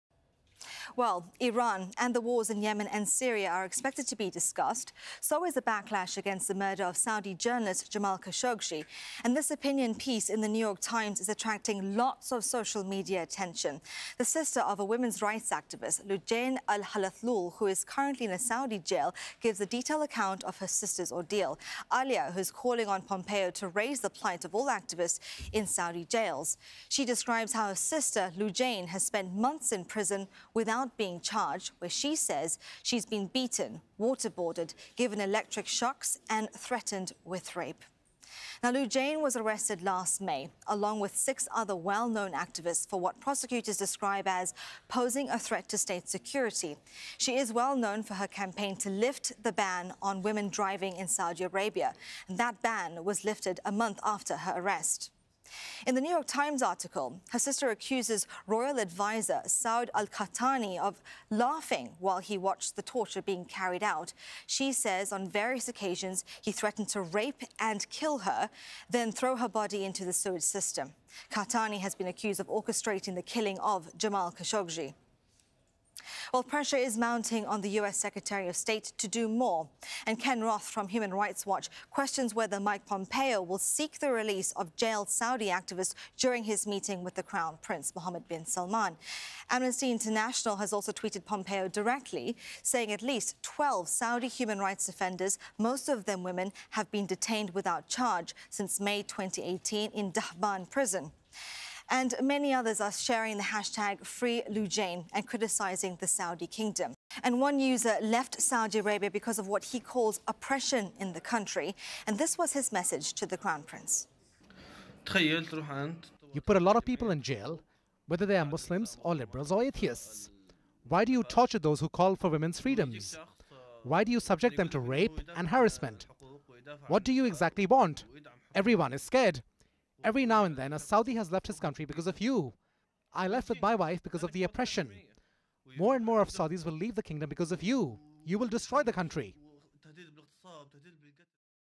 Sister of jailed Saudi female activist calls on Pompeo's help
The U.S. Secretary of State has arrived in Riyadh on the latest leg of his Middle East tour. Mike Pompeo will be holding talks with Saudi Crown Prince Mohammed bin Salman.
He's expected to address a range of issues, many online are hoping human rights are brought up.
Raheela Mahomed and Usama Alony report.